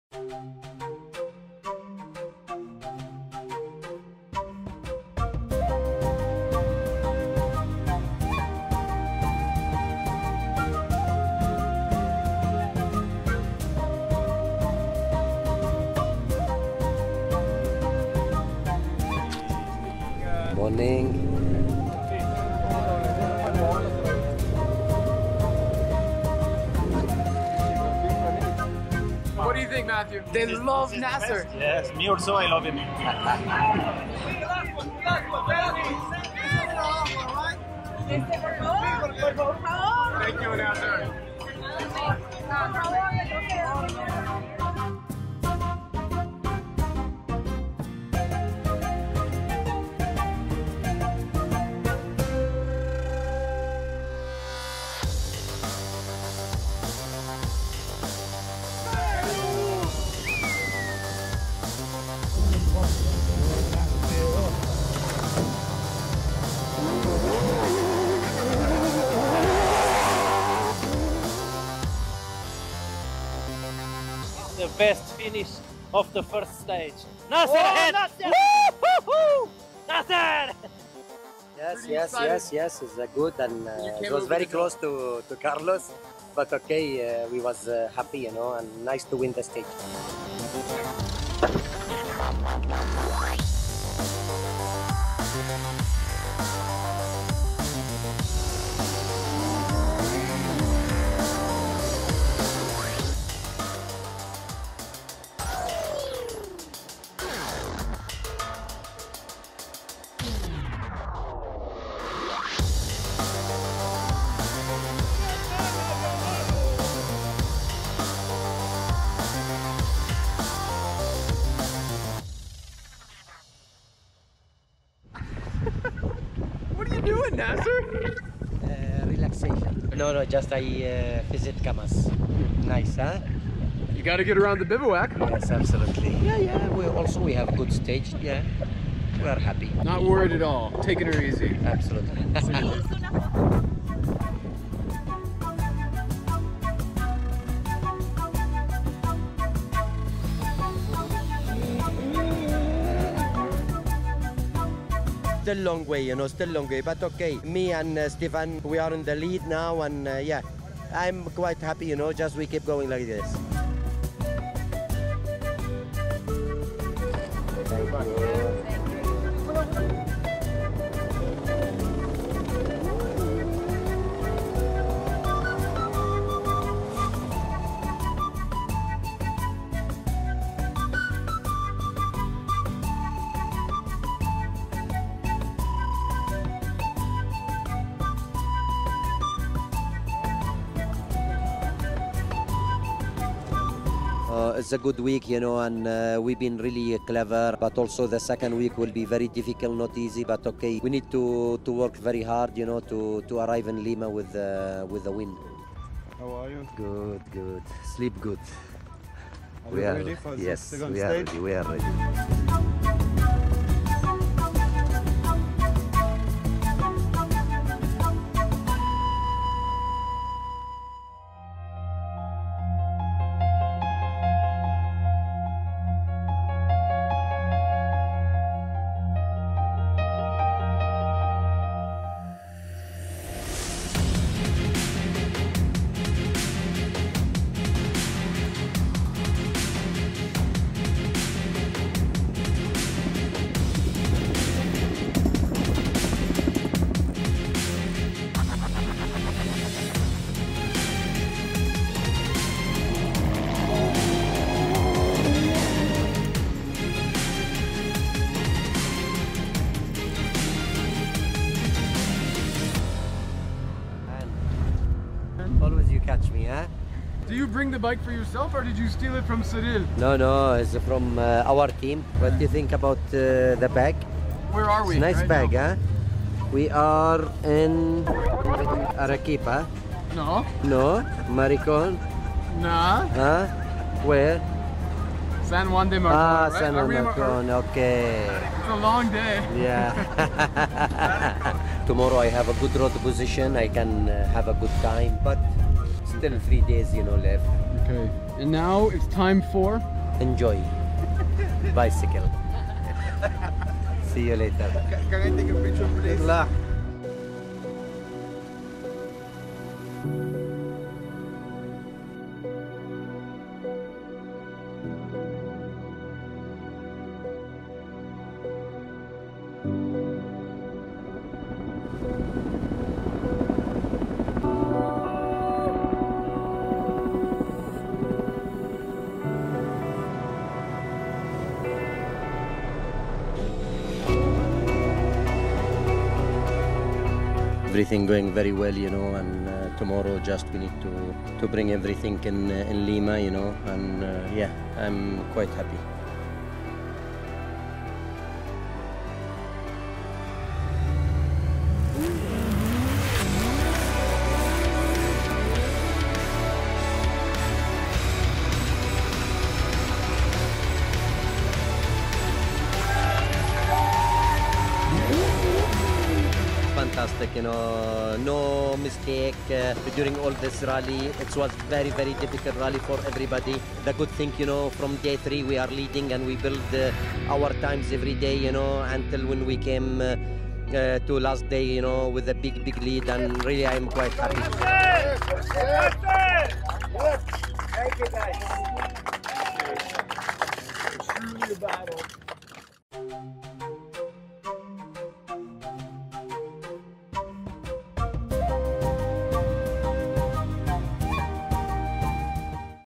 Dakar Preparation Is Over - It's Time To Race! | Destination Peru Part 5
Get into the gruelling action through all 10 stages of the 2019 Dakar Rally with Nasser Al-Attiyah as he works hard to dominate nearly every part of this race. Did his hard work pay off?